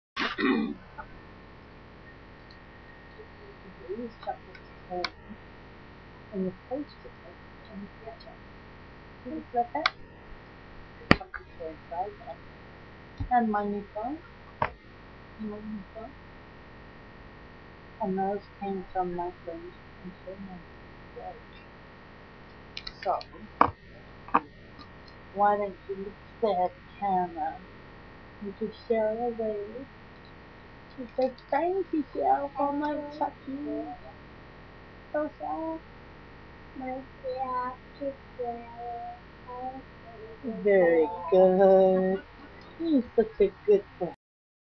prezzie from cheryl
giving kacey his chocolates